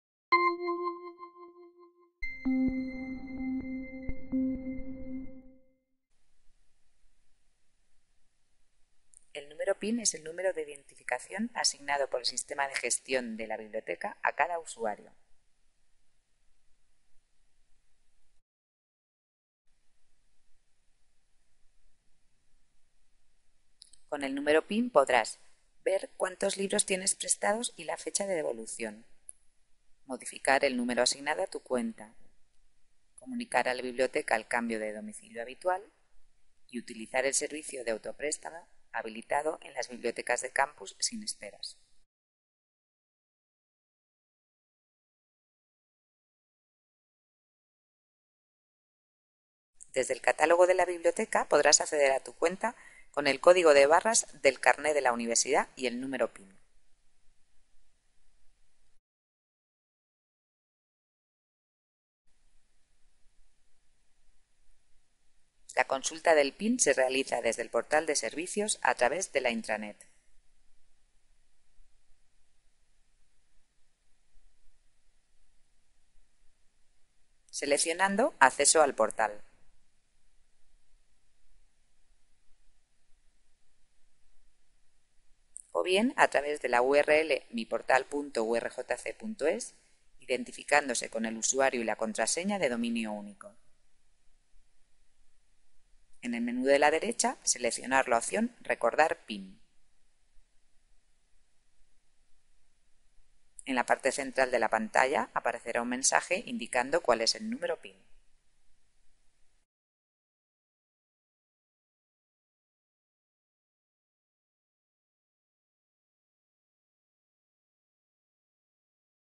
Número PIN de la BURJC
Videotutorial sobre el número PIN de la Biblioteca de la URJC